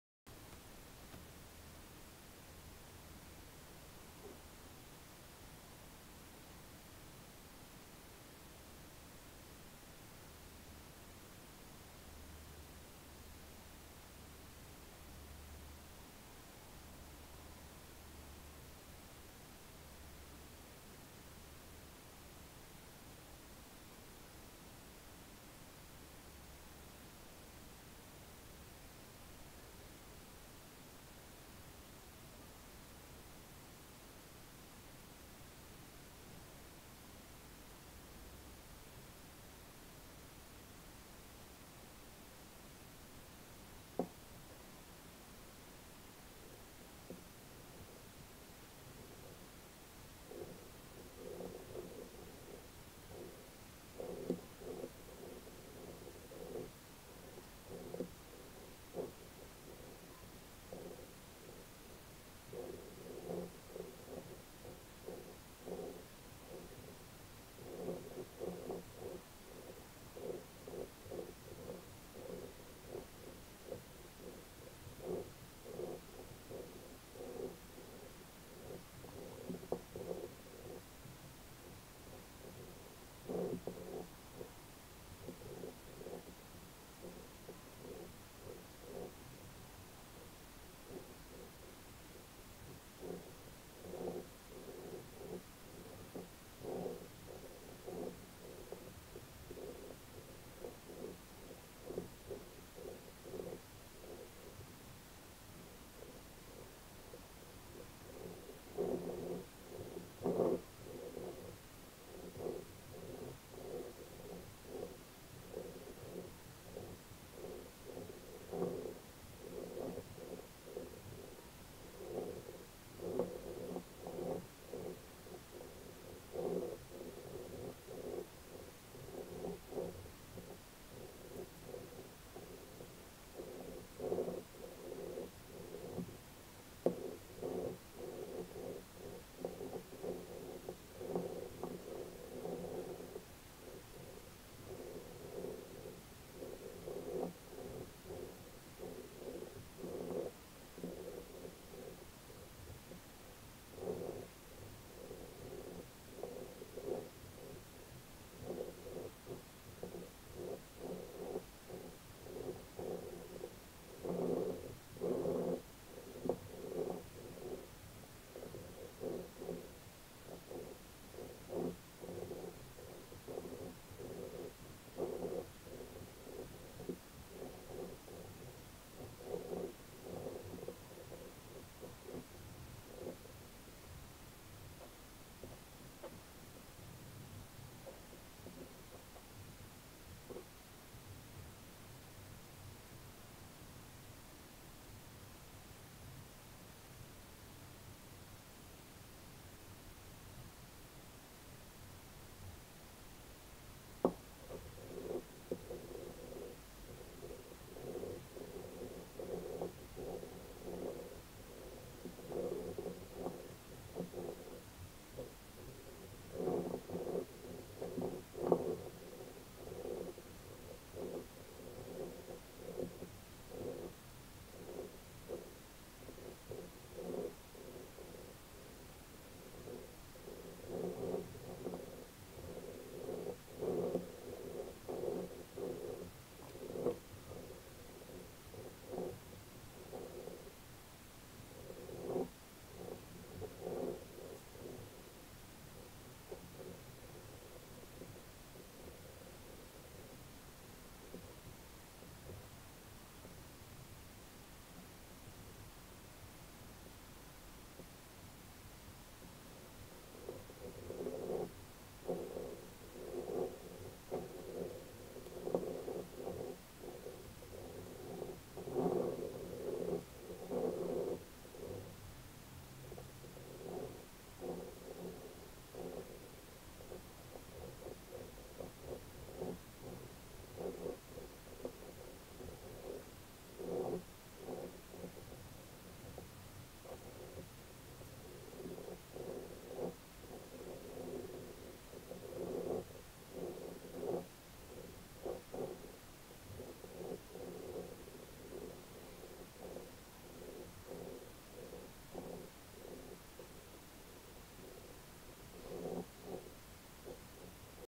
ASMR - sounds (marble rolling on a wooden desktop) and slow hand movements
I would just like to inform you that this video, I don't speak.  I swear I'm not on anything, but I decided to make this simply because I thought the marble looked really pretty with the light behind it.  I hope you all find this soothing!